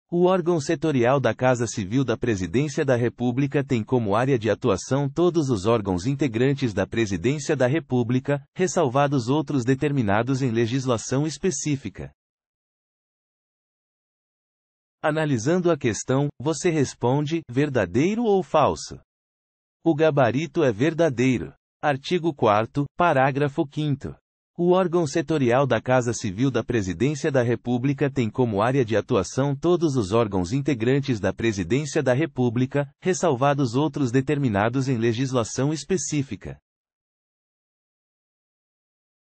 QScon Direito: [Art. 4º, § 5º, Lei 10.180/2001] Marinha 2015 - Segundo Tenente
Art. 4º, § 5º, Lei 10.180/2001 | Marinha 2015 | Segundo Tenente

O órgão setorial da Casa Civil da Presidência da República tem como área de. atuação todos os órgãos integrantes da Presidência da República, ressalvados. outros determinados em legislação específica.

Gabarito: Verdadeiro